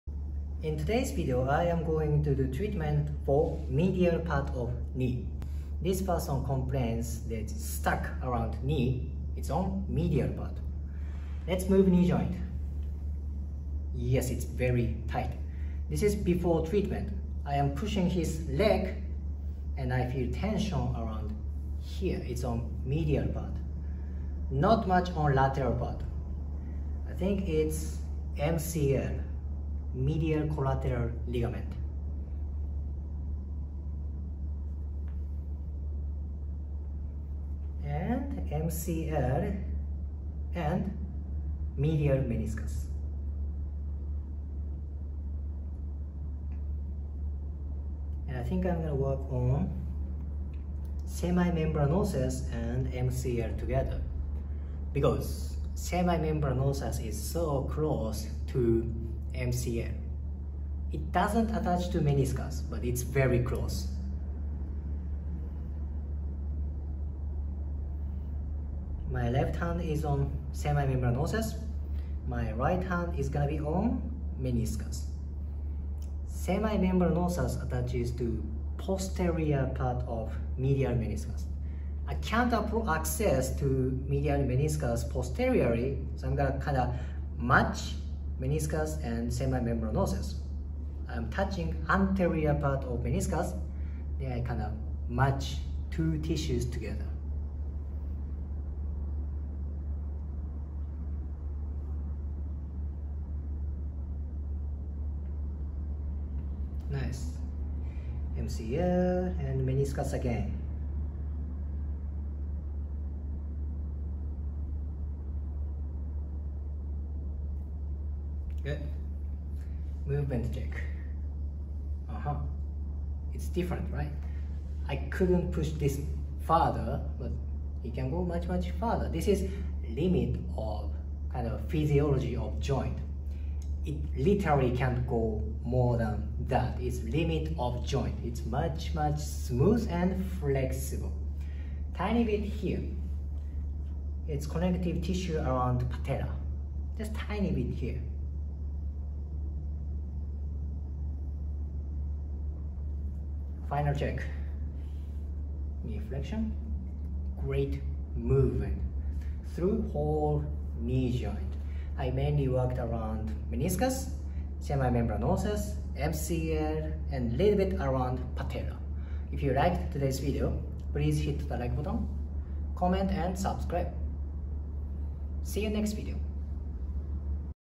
Manual therapy for inside of knee
At 1:00, I said "Semimembranosus doesn't attach to meniscus." Yes it attaches there. I meant it doesn't attach to MCL.